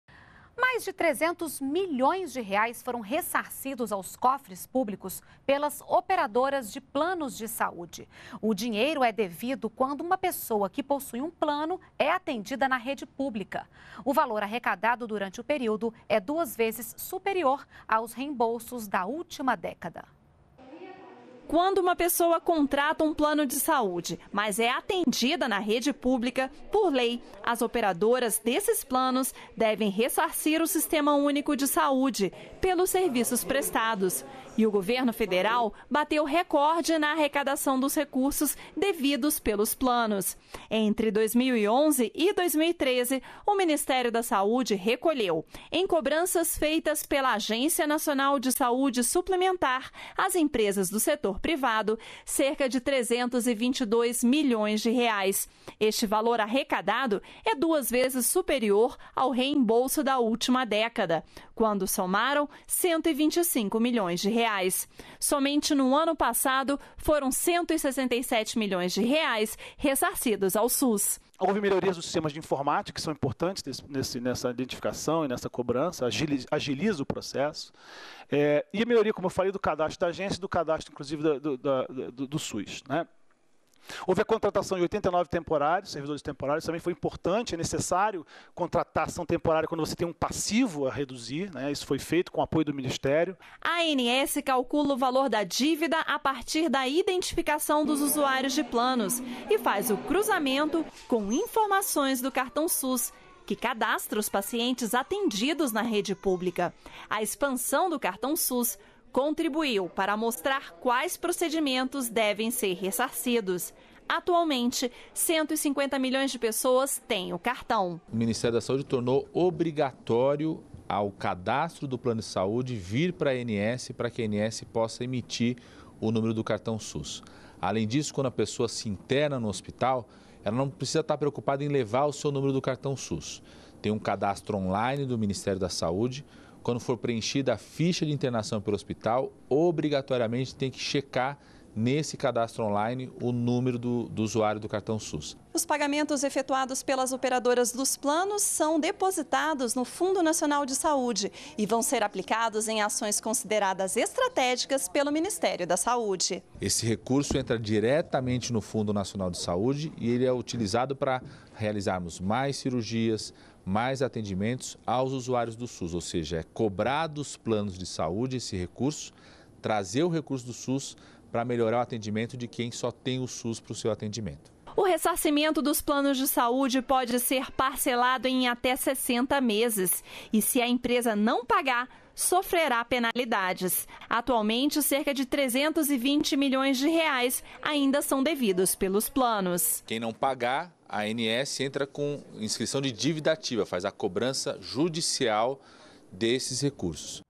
Ressarcimento pago ao governo pelas operadoras de planos de saúde tem valor recorde
NBR NOTÍCIAS - 09.01.14: Nos últimos três anos, o governo teve o ressarcimento de R$ 322 milhões de operadoras de planos de saúde. O dinheiro é devido quando uma pessoa que possui plano é atendida na rede pública. O valor arrecadado durante o período é duas vezes superior aos reembolsos da última década. Em 2013, o ressarcimento foi de R$ 167 milhões. Além de aumentar o volume de recursos reembolsados, a Agência Nacional de Saúde Suplementar (ANS) intensificou a cobrança sobre as operadoras. Desde 2011, foram cobradas 483 mil internações, 36% a mais do que na década anterior. Apenas em 2013 foram cobradas 237 mil internações. Os pagamentos efetuados para a Agência são repassados ao Fundo Nacional de Saúde (FNS) e aplicados em ações de saúde e programas estratégicos do Ministério da Saúde.